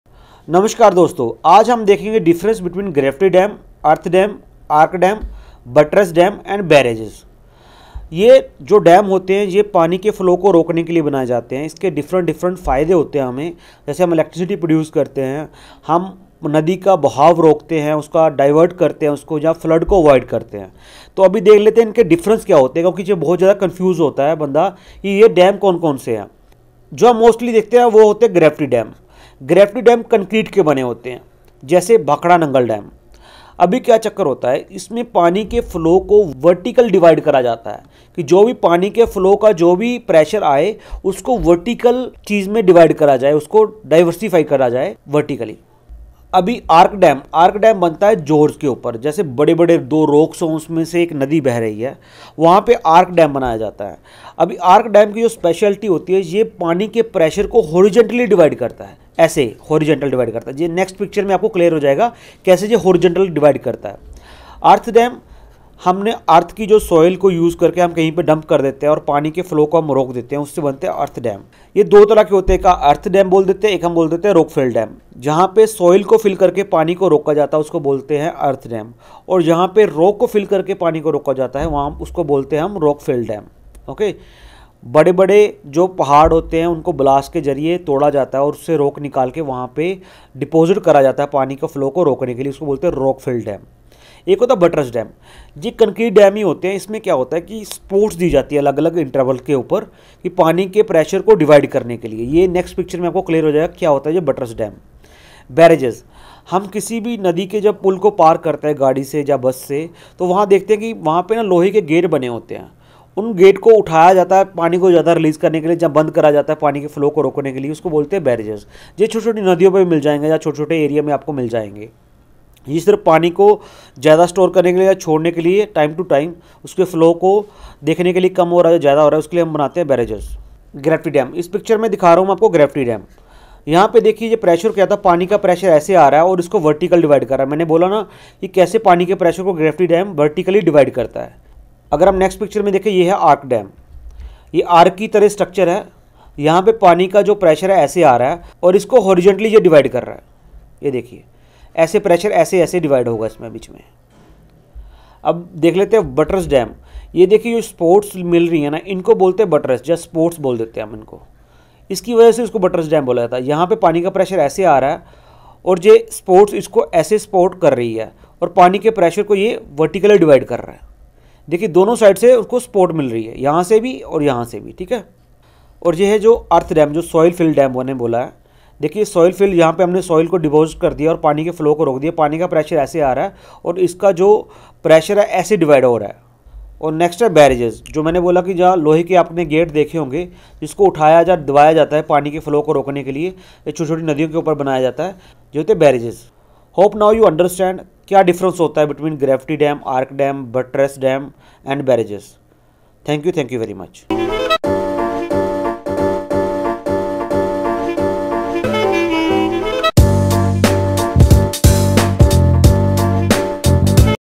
What is difference between Gravity dam ,Earth dam,Arch dam,Buttress Dam,Barrage | Hindi | Quikr Exam
Gravity Dam

A gravity dam is a dam constructed from concrete or stone masonry and designed to hold back water by primarily utilising the weight of the material alone to resist the horizontal pressure of water pushing against it.

Arch Dam

Arch dams, where the pressure of the water is “transmitted” to some solid structure, usually built in a gorge.

Earth Dam (Embankment dams)

dams are made from compacted
Earth,

Some dam are rock filled instead of earth soil hence called rock filled dam

Buttress Dam

A buttress dam or hollow dam is a dam with a solid, water-tight upstream side that is supported at intervals on the downstream side by a series of buttresses or supports.

Barrage

A barrage dam is a special kind of dam which consists of a line of large gates that can be opened or closed to control the amount of water passing the dam